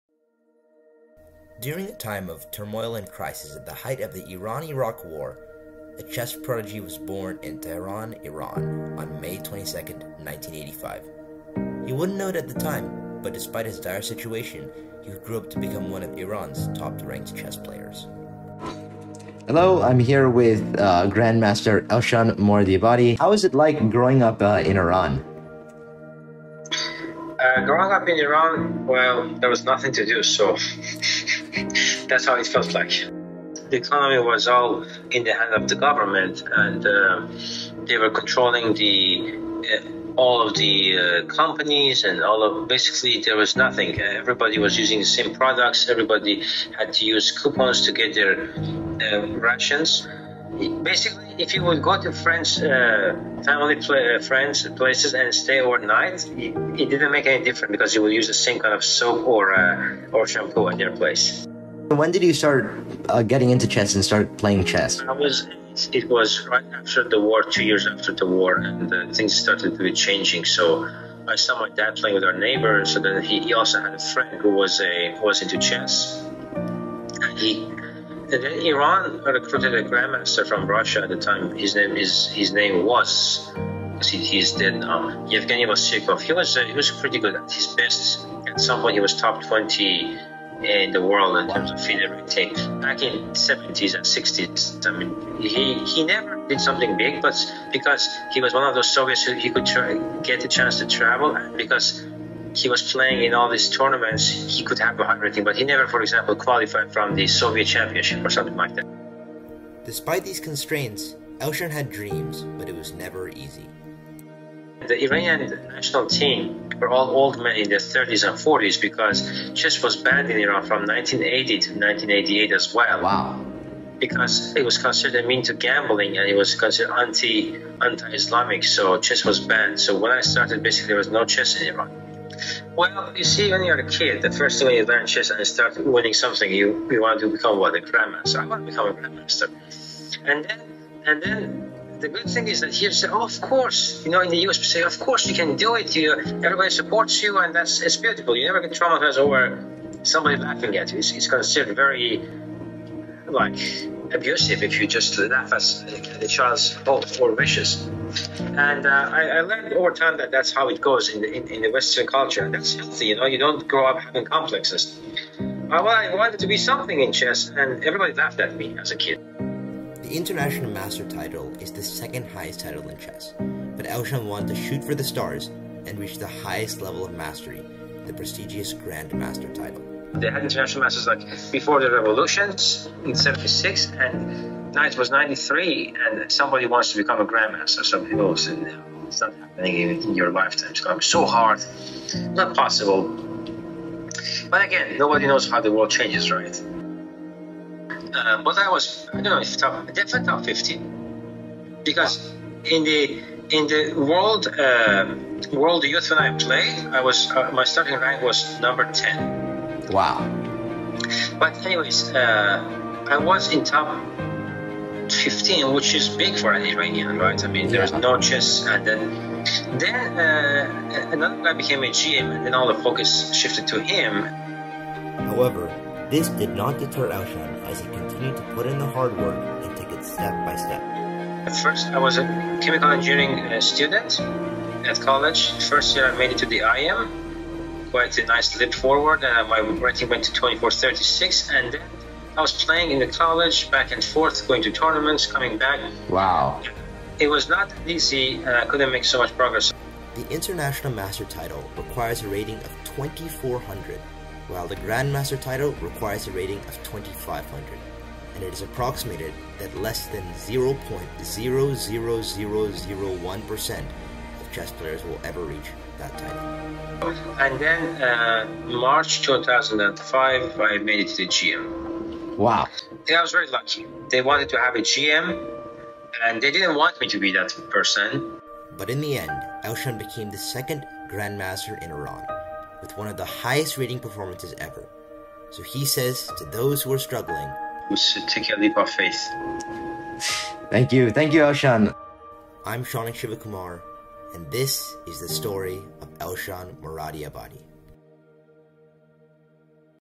Resilient Minds - Elshan's Story
I am thrilled to share my podcast with Elshan Moradiabadi, a chess grandmaster. He shares his childhood story which takes place in a time of war and revolution in Iran and how he worked hard and persevered to become a Grandmaster, the highest title in chess. His story highlights the struggle of those around the world working hard to achieve their goals and it connects to my core values of perseverance and determination by inspiring and empowering others to never give up despite the challenges they face. This story serves as a reminder that pressure makes diamonds, and that those who are passionate and work hard, no matter the environment they are brought up in, can reach for the stars and achieve their dreams.

Do you have any inspiring personal stories to share with the world?